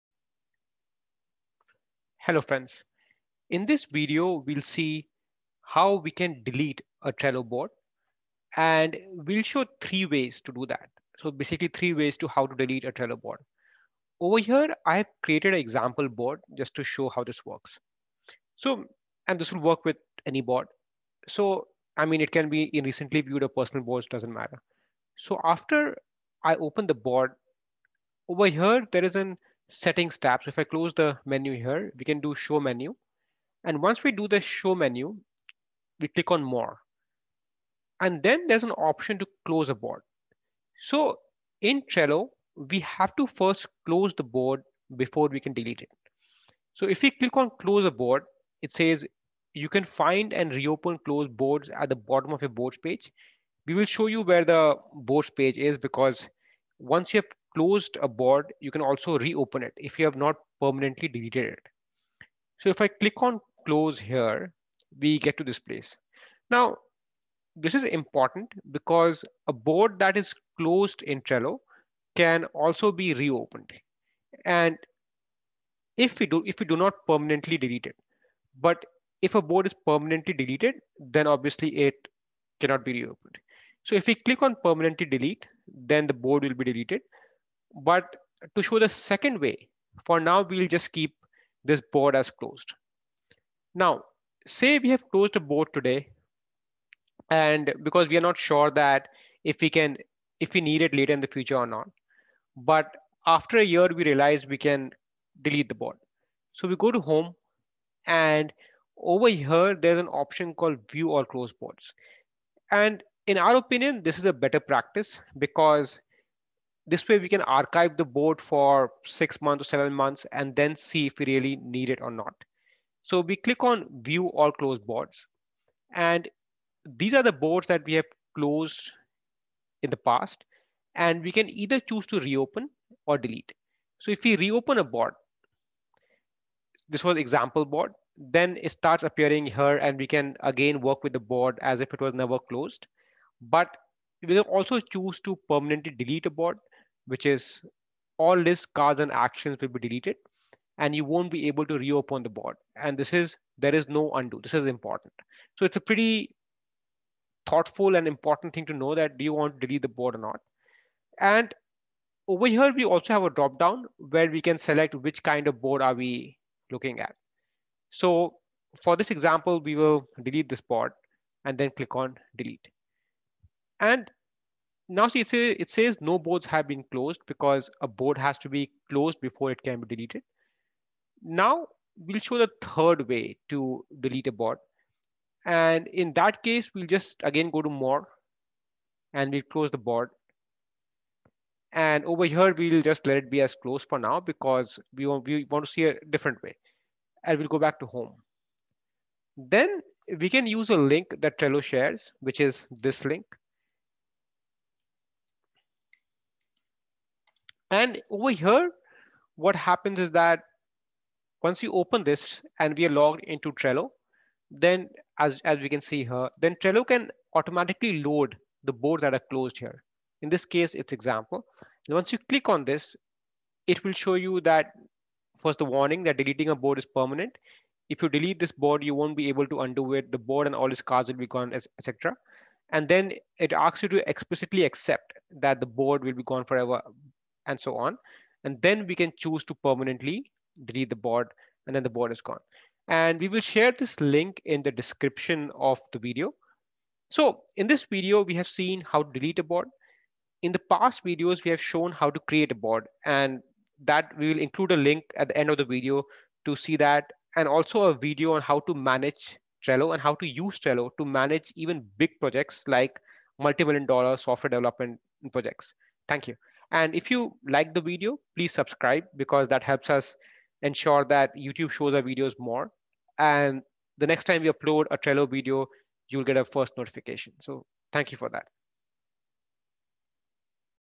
Trello 2022: How to delete a board in Trello
This video shows how to delete a board in Trello. Trello is a excellent software that can be used to manage business projects, software developement. This video shows how to delete a board in Trello. There are 3 ways to delete a board in Trello and this video shows all ways to delete a board in Trello!

Trello is a web-based, Kanban-style, list-making application . It can be used for project management, task management, business management. This is trello tutorial